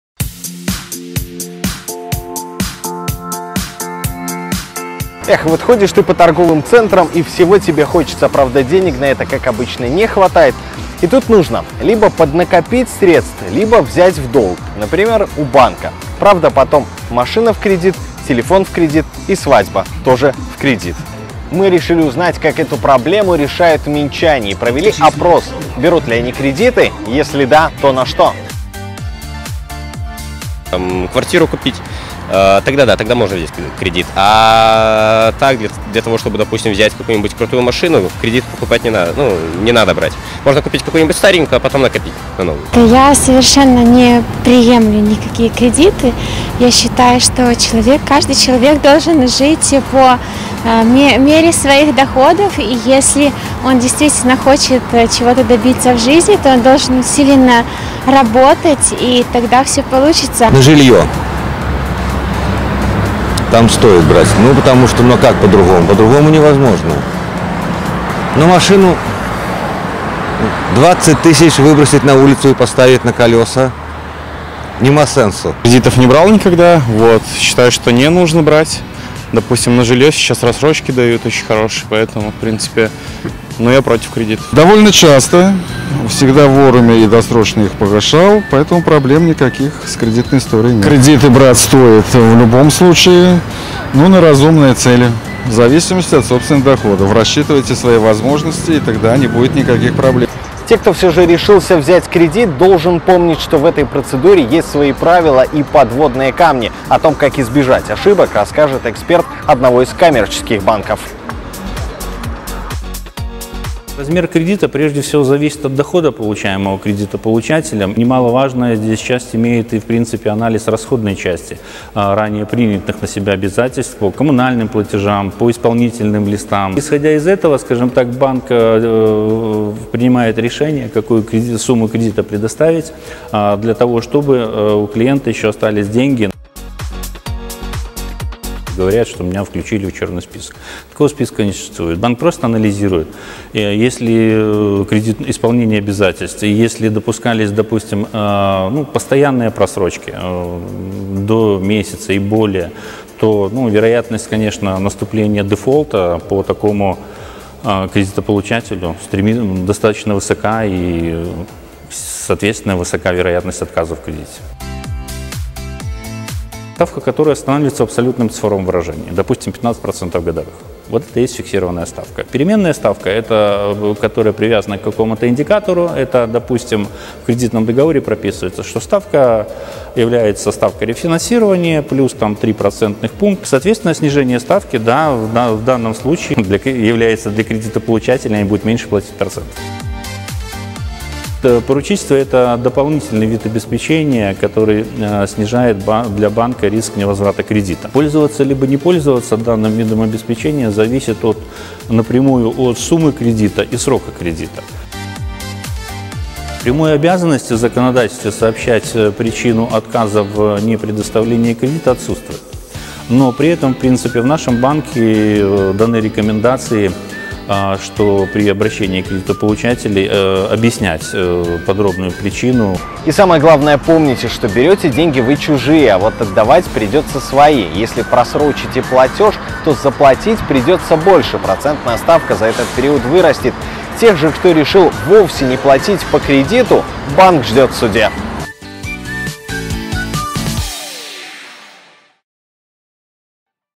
Кредит — добро или зло? Показываем все подводные камни!
Как часто наши соотечественники берут кредиты? С чем они сталкиваются?

Порталу в этих вопросах помог разобраться эксперт - заместитель директора Департамента развития розничных продуктов и цифровизации Белагропромбанка Игорь Сукора.

Предлагаем вашему вниманию сюжет корреспондента Алексея Немкевича.